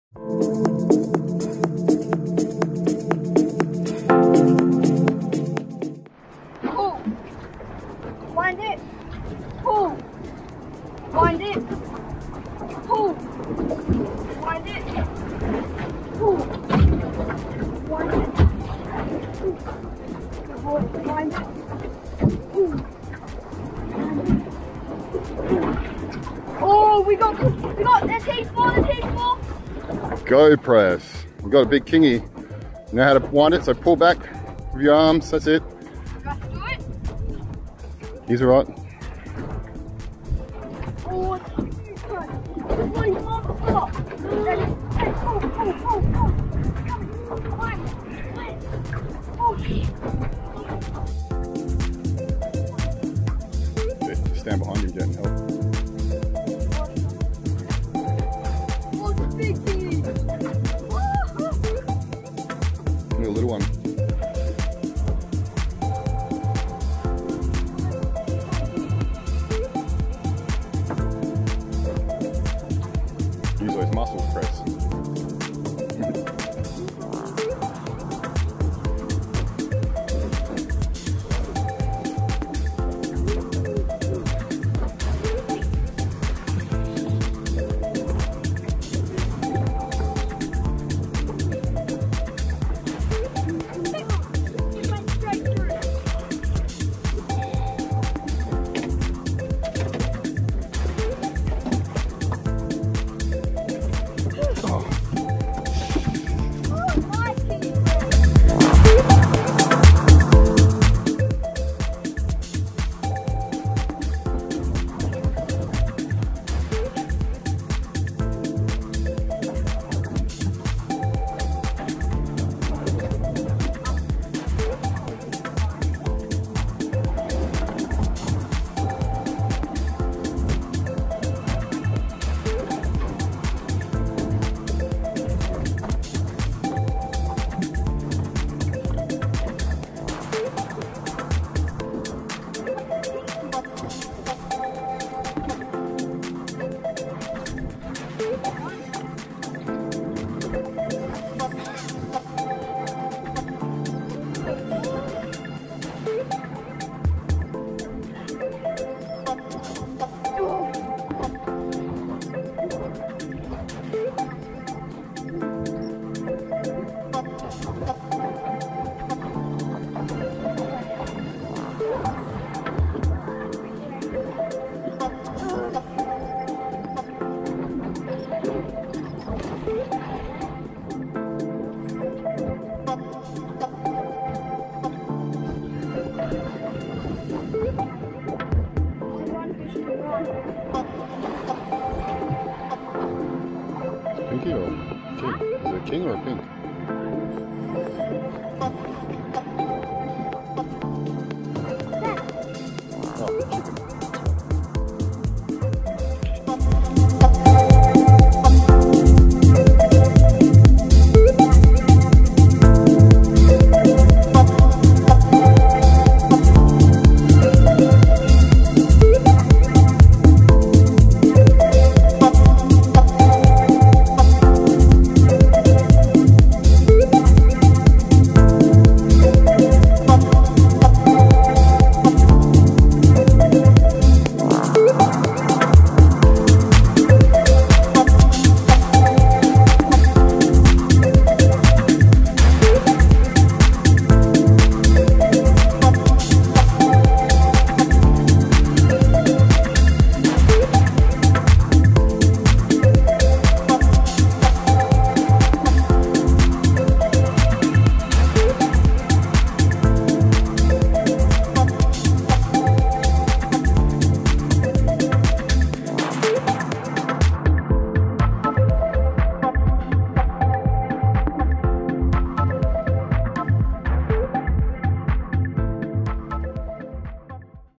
Kingfish off Perth western Australia. Prestons first
Managed to get 4 fish in 30min before his sea sickness kicked in